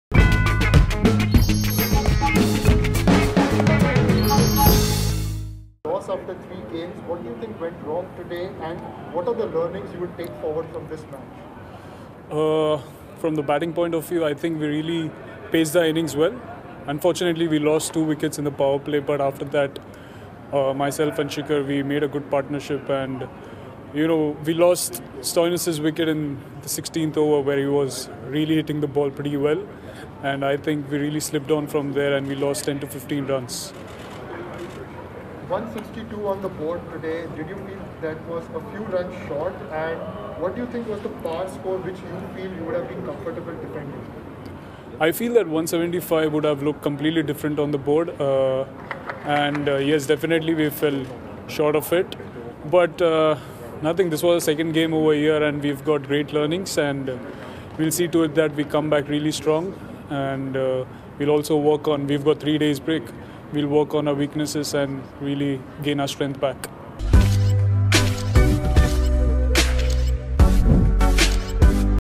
IPL 2020: We'll come back strong, says Shreyas Iyer after losing match against MI | Oneindia Sports
Delhi Capitals captain Shreyas Iyer after losing match against Mumbai Indians said that the team will come back strong. Shreyas Iyer said From batting point of view I think we really paced the innings well. Unfortunately we lost 2 wickets in power play but after that Shikhar and I made a good partnership.

कोरोना महामारी के बीच इंडियन प्रीमियर लीग में रविवार को डबल हेडर का दूसरा मुकाबला मुंबई इंडियंस और दिल्ली कैपिटल्स के बीच खेला गया। जिसमें दिल्ली कैपिटल्स ने टॉस जीतने के बाद पहले बल्लेबाजी का फैसला किया और मुंबई के सामने 163 रनों का लक्ष्य रखा। जिसके जवाब में मुंबई ने सलामी बल्लेबाज क्विंटन डी कॉक और सूर्य कुमार यादव की पारी के दमपर आसानी से जीत हासिल कर ली।